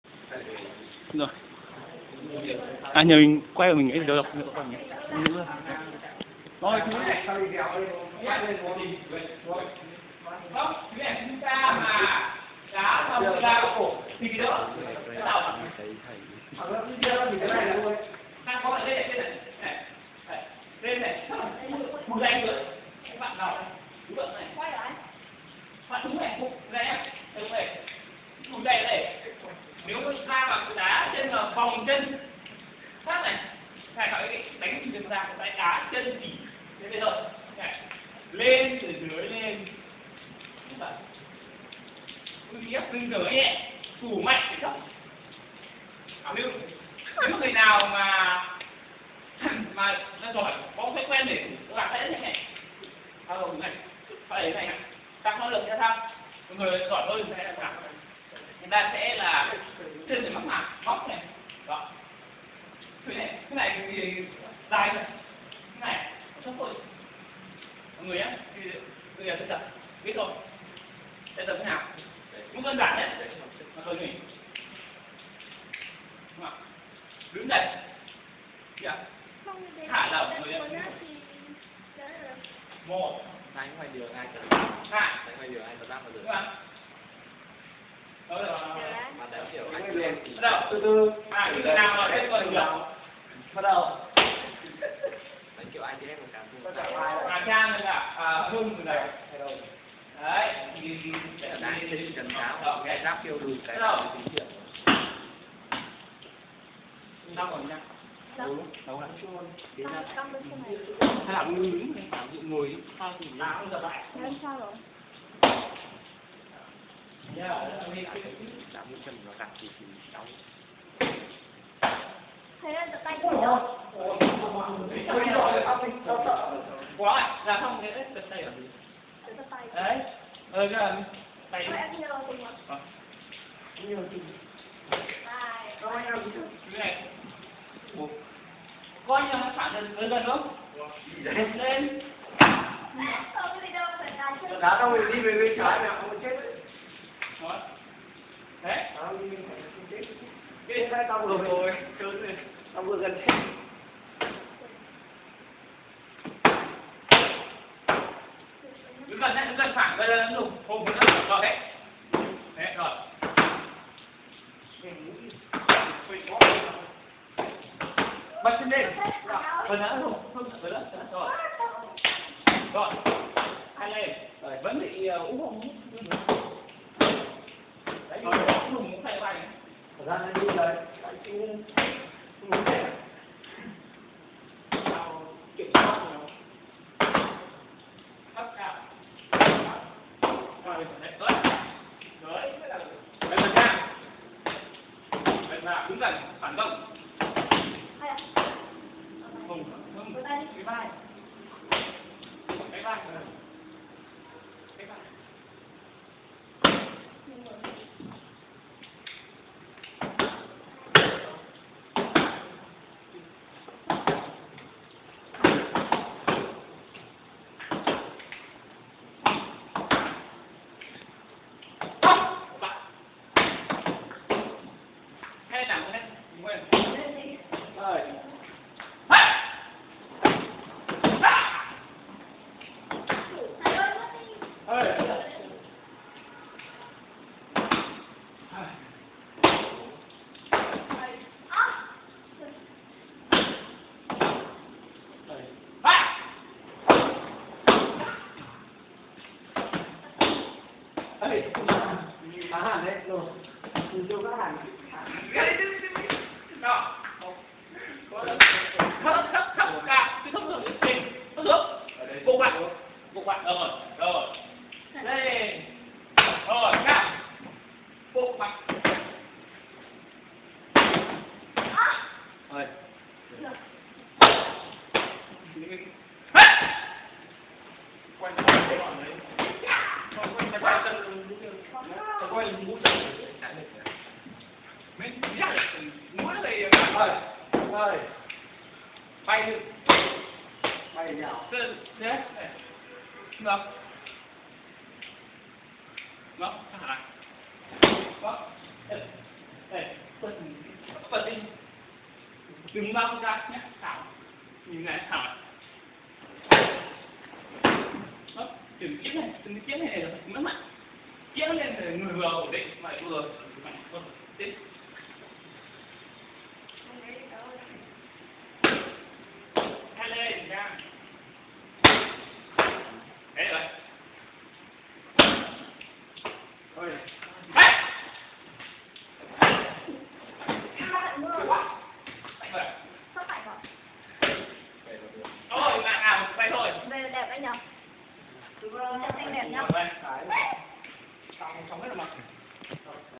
CLB Taekwondo ĐH Thăng Long thứ 2 23-7-2012 tại cửa thư viện 1
moto_26475.3gp
tập chân
cầm đích Trình 178
người tập Trang 179,, Trang 250, Hưng 96, Hùng 101
quay phim Tùng 148 và Lộc thiếu lâm